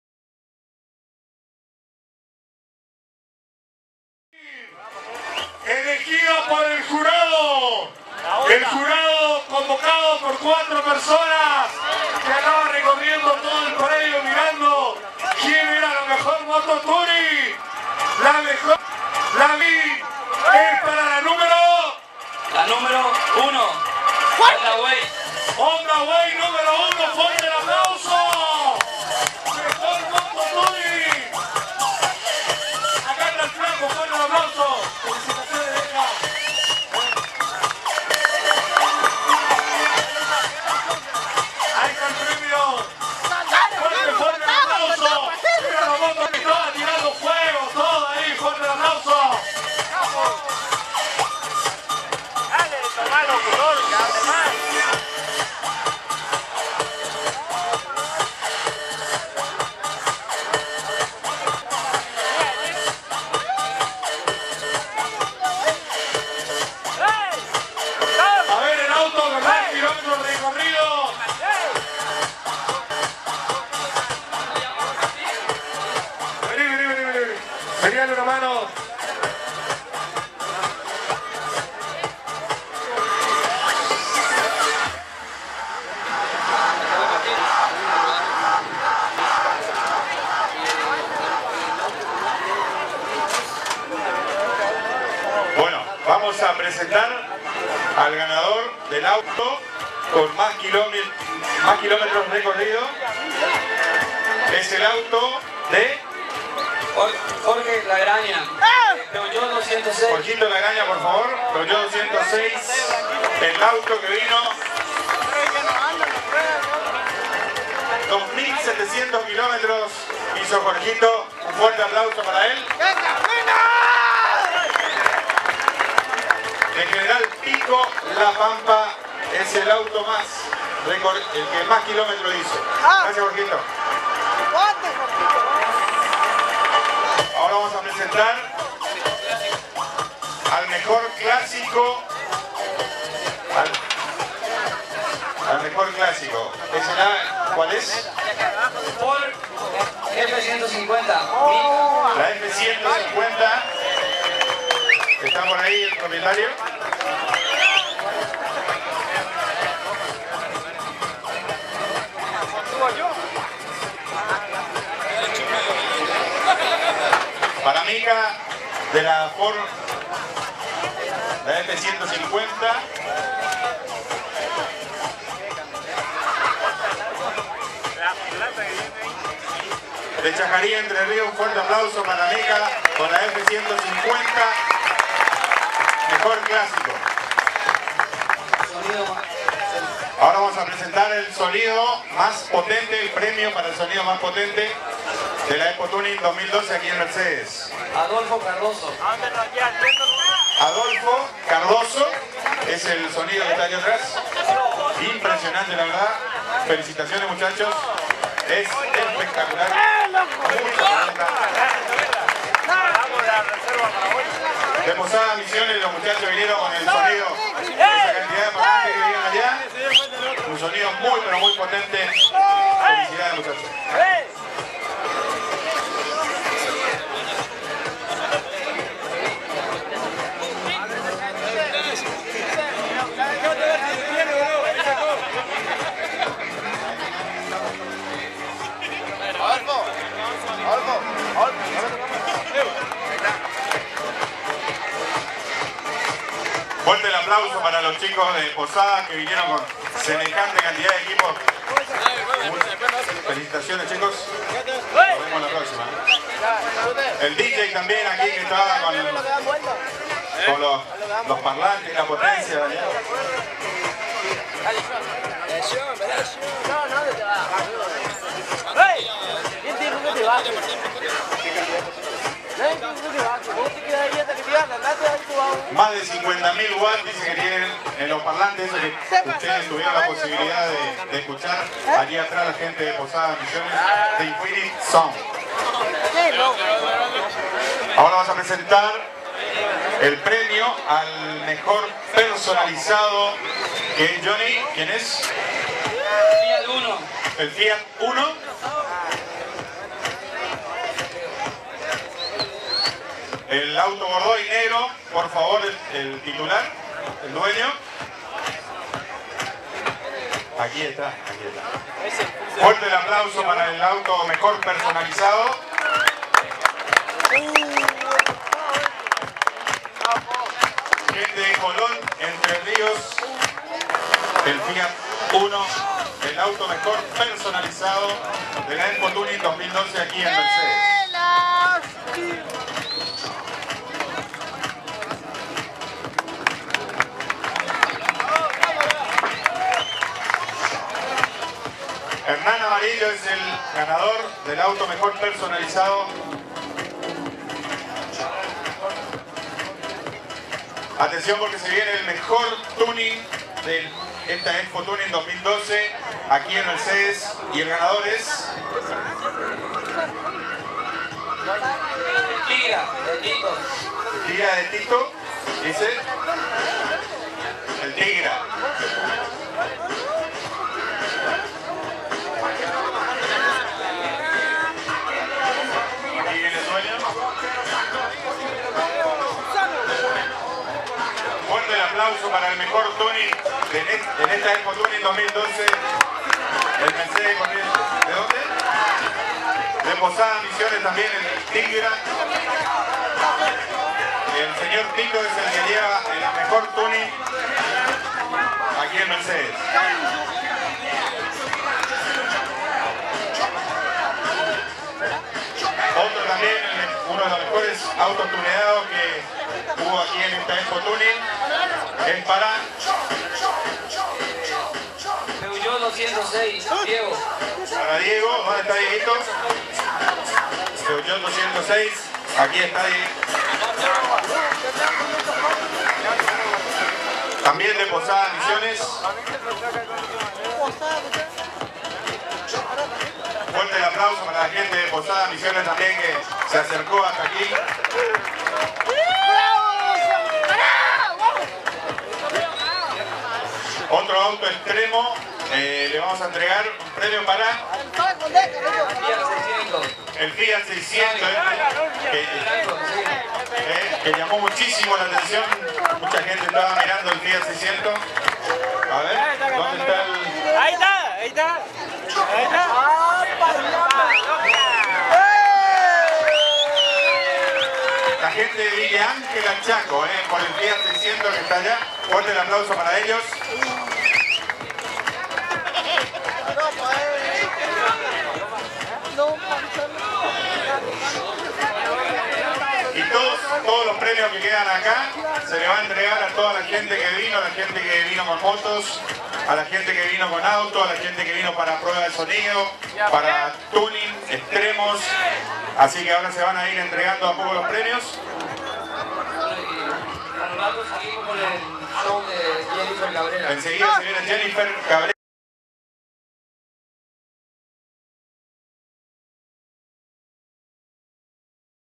Jurados Eleccion Reina Coronacion Expo Tuning.mp4
Coronacion de la chica Tuning de la ciudad de Mercedes